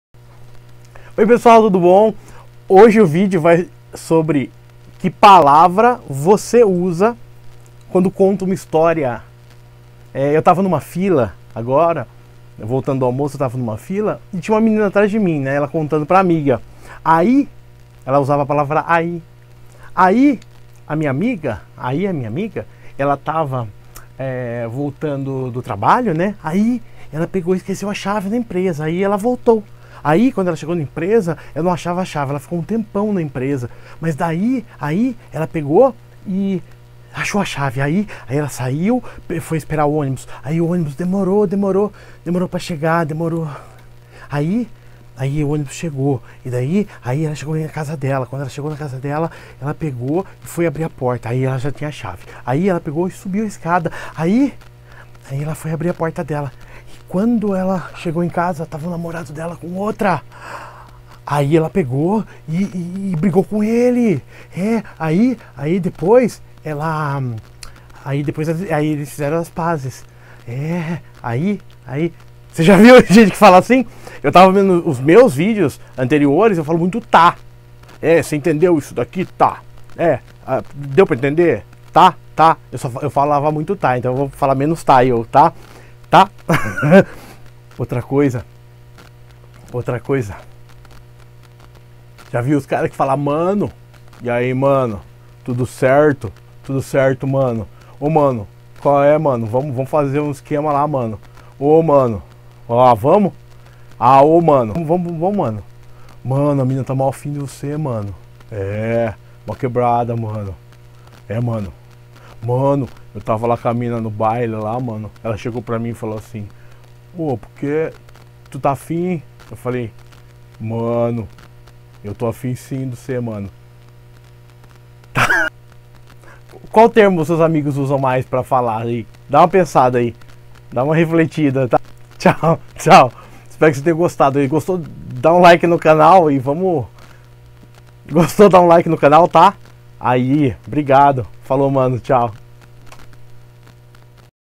QUE PALAVRA VOCÊ USA PARA CONTAR UMA HISTÓRIA?
Humor e reflexão sobre como contamos as histórias, ao relatar um fato. Isso também mostra o estilo de cada compositor.